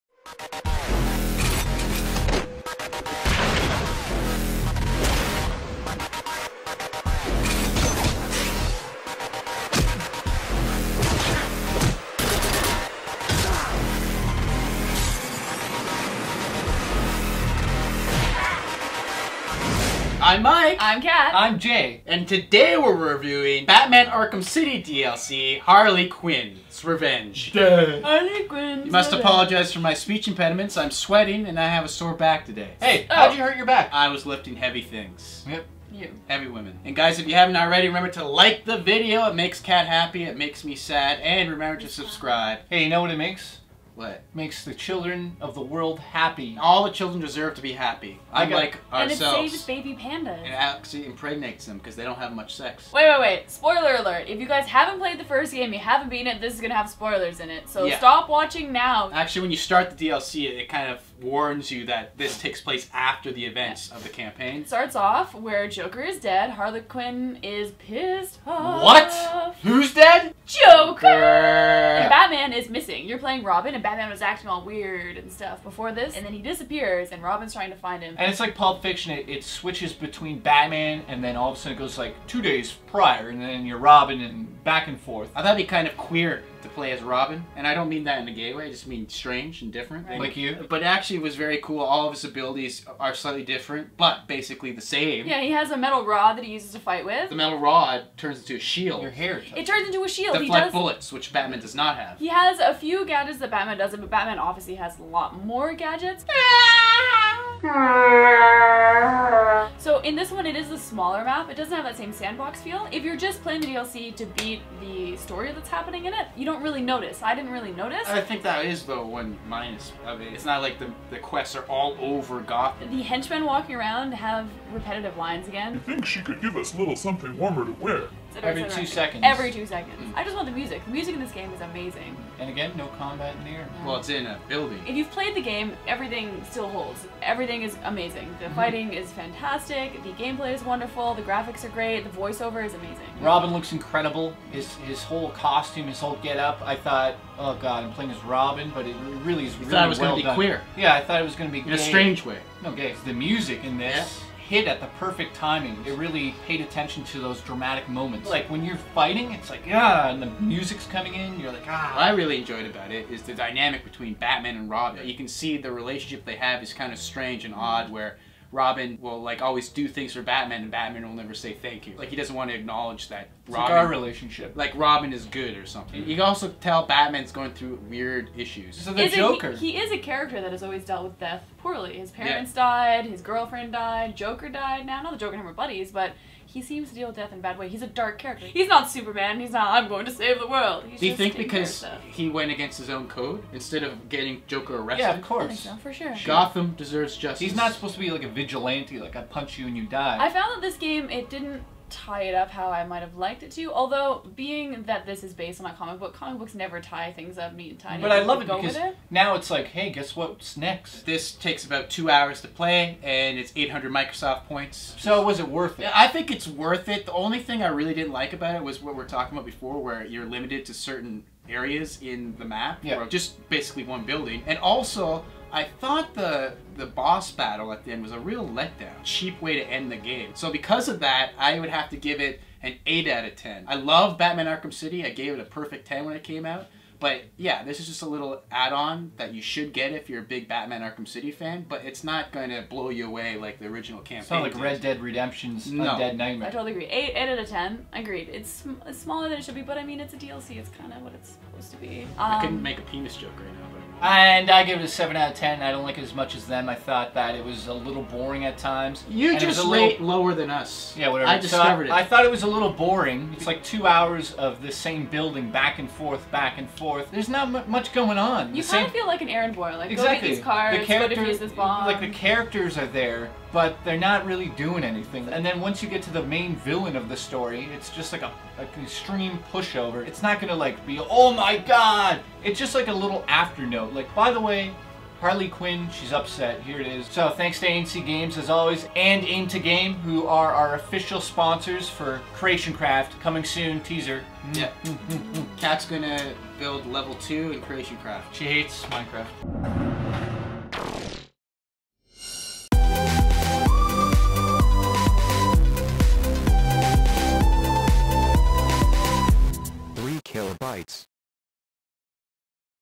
Harley Quinn's Revenge Review - Batman: Arkham City DLC
3_KILLA_BYTES have returned to Gotham City for this super sexy review of the BATMAN ARKHAM CITY DLC: HARLEY QUINN'S REVENGE. Find out if this download directly from the bat cave is worth the money or the 800 Microsoft points. In this episode of 3KB Kat turns into a man, Jay slows down and Mike can't make a penis joke. Join the fun with the 3KB crew and fight crime evading and using your counter attack an you just might fall into Harley's cleavage.